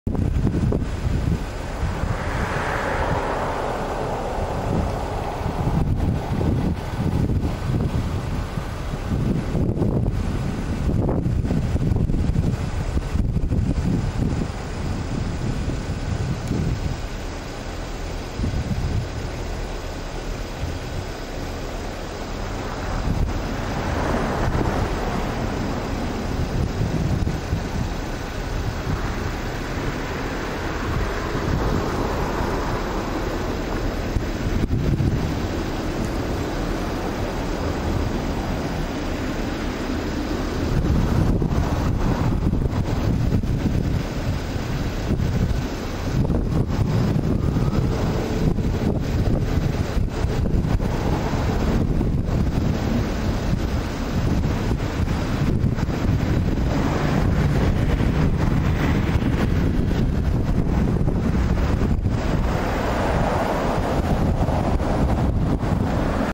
Schneechaos sorgt für Stillstand auf der Autobahn A72!
Freistaat Sachsen - Treuen A72: Wegen des starken Schneefalls und Schneeverwehungen ist der Verkehr auf der Aurobahn A72 in Sachsen am Montag nahezu komplett zum Erliegen gekommen.